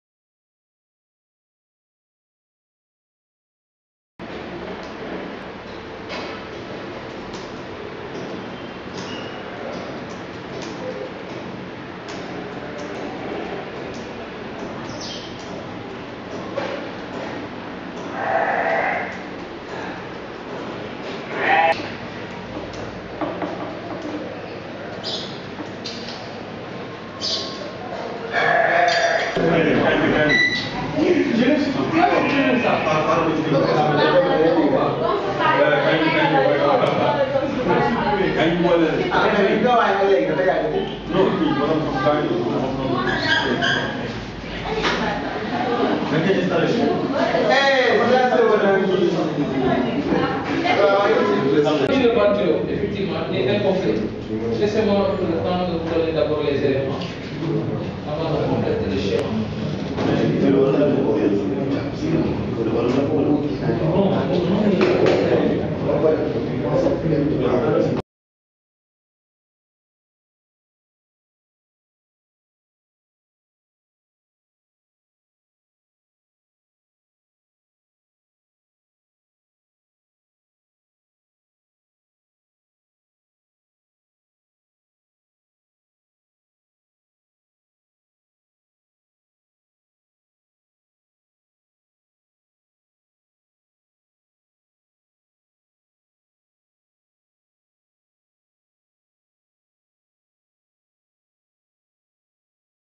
SCHOOL CTI - v1
Yousouf BALDE's video of his school (CTI) in Dakar, Senegal.